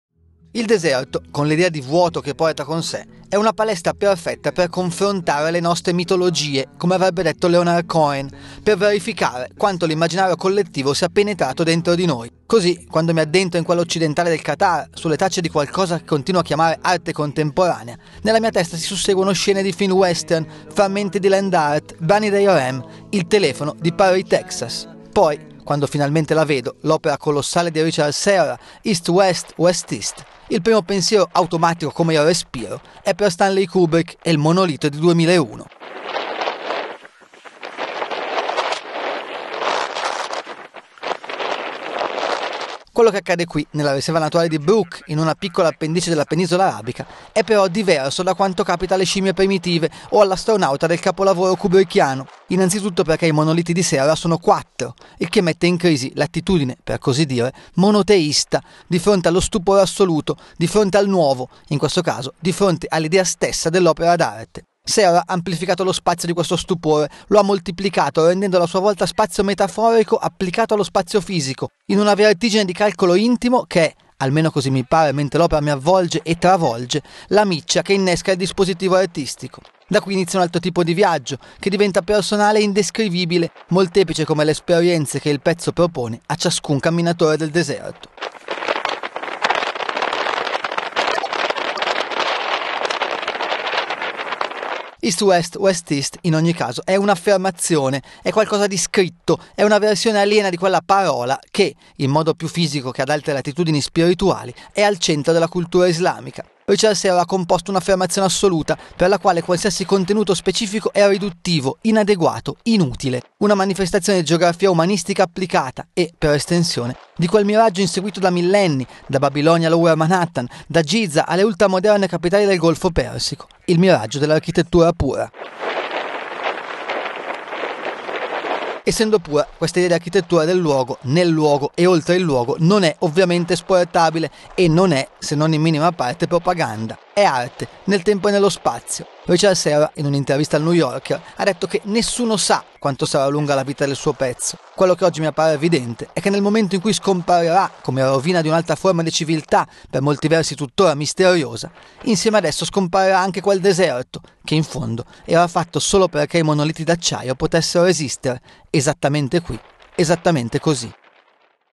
Arte nel tempo, come architettura pura: Richard Serra in Qatar
Doha (askanews) - Il deserto, con l'idea di vuoto che porta con sé, è una palestra perfetta per confrontare le nostre mitologie, come avrebbe detto Leonard Cohen, per verificare quanto l'immaginario collettivo sia penetrato dentro di noi. Così quando mi addentro in quello occidentale del Qatar, sulle tracce di qualcosa che continuo a chiamare "arte contemporanea", nella mia testa si susseguono scene di film western, frammenti di Land Art, brani dei R.E.M, il telefono di "Paris, Texas".
Poi, quando finalmente la vedo, l'opera colossale di Richard Serra "East-West/West-East", il primo pensiero, automatico come il respiro, è per Stanley Kubrick e il Monolito di "2001".
Quello che accade qui, nella riserva naturale di Brouq, in una piccola appendice della penisola arabica, è però diverso da quanto capita alle scimmie primitive o all'astronauta del capolavoro kubrickiano. Innanzitutto perché i monoliti di Serra sono quattro, il che mette in crisi l'attitudine, per così dire, "monoteista" di fronte allo stupore assoluto, di fronte al nuovo, in questo caso di fronte all'idea stessa dell'opera d'arte.
Serra ha amplificato lo spazio di questo stupore, lo ha moltiplicato rendendolo a sua volta spazio metaforico applicato allo spazio fisico, in una vertigine di calcolo intimo che è, almeno così mi pare mentre l'opera mi avvolge e travolge, la miccia che innesca il dispositivo artistico. Da qui inizia un altro tipo di viaggio, che diventa personale e indescrivibile, molteplice come le esperienze che il pezzo propone a ciascun camminatore del deserto.
"East-West/West-East", in ogni caso, è una affermazione, è qualcosa di "scritto", è una versione aliena di quella "parola" che è, in modo più fisico che ad altre latitudini spirituali, al centro della cultura islamica. Richard Serra ha composto un'affermazione assoluta, per la quale qualsiasi contenuto specifico è riduttivo, inadeguato, inutile. Una manifestazione di geografia umanistica applicata, e, per estensione, di quel miraggio inseguito da millenni, da Babilonia a Lower Manhattan, da Giza alle ultramoderne capitali del Golfo Persico: il miraggio dell'architettura pura.
Essendo pura, questa idea di architettura del luogo, nel luogo e oltre il luogo, non è ovviamente esportabile, e non è, se non in minima parte, propaganda. È arte, nel tempo e nello spazio. Richard Serra, in un'intervista al New Yorker ha detto che "nessuno sa" quanto sarà lunga la vita del suo pezzo. Quello che oggi mi appare evidente è che nel momento in cui scomparirà, come rovina di un'altra forma di civiltà - per molti versi tuttora misteriosa - insieme a esso scomparirà anche quel deserto, che in fondo era fatto solo perché i monoliti d'acciaio potessero esistere, esattamente qui, esattamente così.